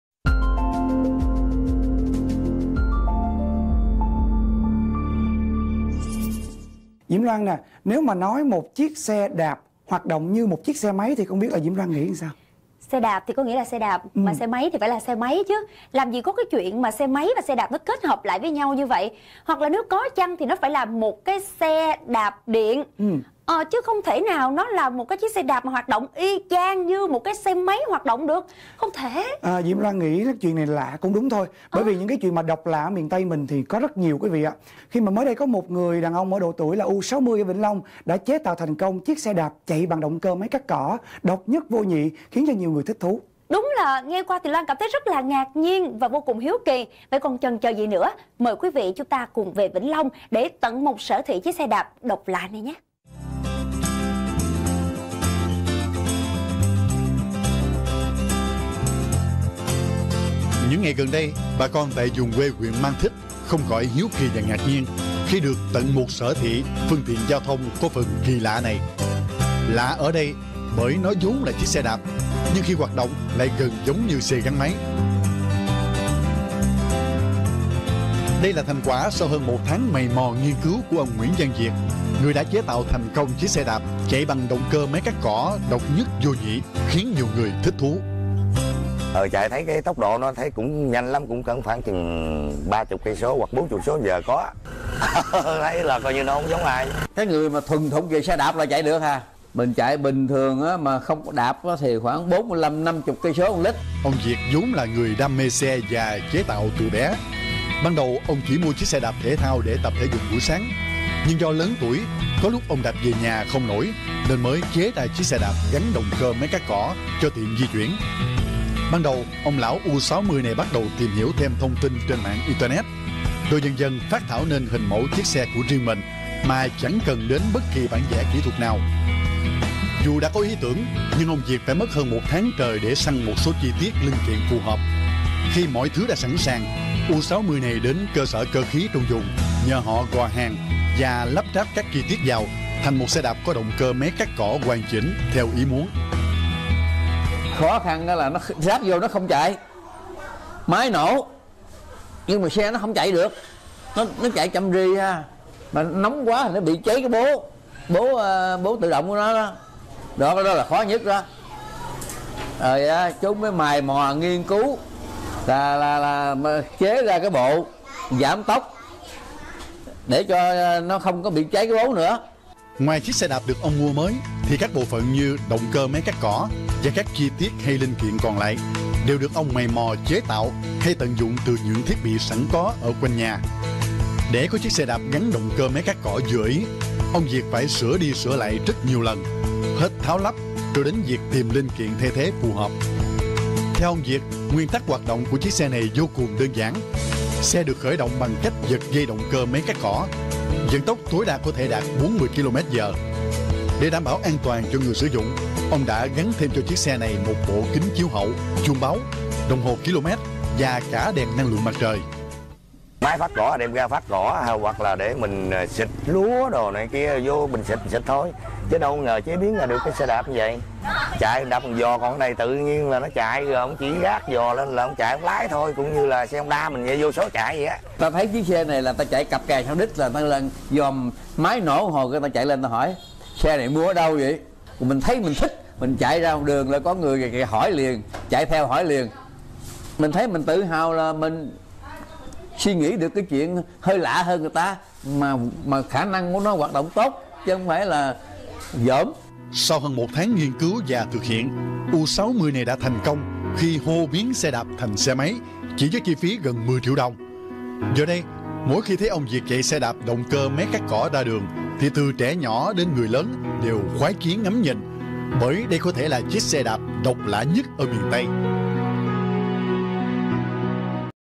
Biến xe đạp thành xe máy với máy cắt cỏ ở Miền Tây
Biến xe đạp thành xe máy với máy cắt cỏ của một bác ở Vĩnh Long